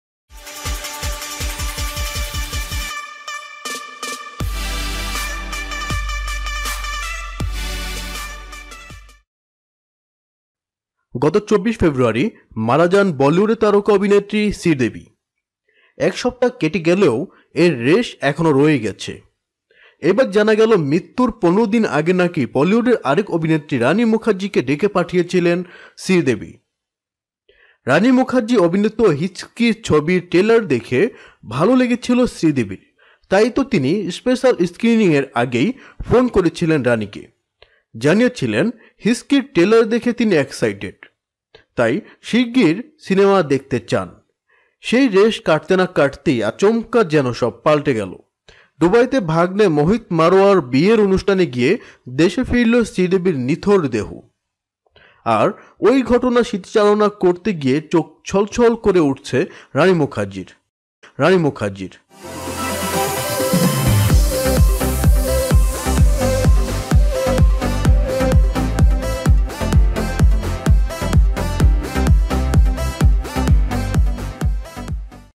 মৃত্যুর ১৫ দিন আগে কেন রানিকে ডেকে পাঠিয়েছিলেন শ্রীদেবী? শ্রীদেবীর মৃত্যুর আসল রহস্য দেখুন
গত ২৪ ফেব্রুয়ারি মারা যান বলিউডের তারকা অভিনেত্রী শ্রীদেবী। এক সপ্তাহ কেটে গেলেও এর রেশ এখনও রয়ে গেছে। এবার জানা গেল মৃত্যুর ১৫ দিন আগে নাকি বলিউডের আরেক অভিনেত্রী রানি মুখার্জি ডেকে পাঠিয়েছিলেন শ্রীদেবী।

জি নিউজের খবর, রানি মুখার্জি অভিনীত ‘হিচকি’ ছবির ট্রেলার দেখে ভালো লেগেছিল শ্রীদেবীর। তাই তো তিনি স্পেশাল স্ক্রিনিংয়ের আগেই ফোন করেছিলেন রানিকে। জানিয়েছিলেন, ‘হিচকি’-র ট্রেলার দেখে তিনি 'এক্সসাইটেড'। তাই শিগগির সিনেমাও দেখতে চান।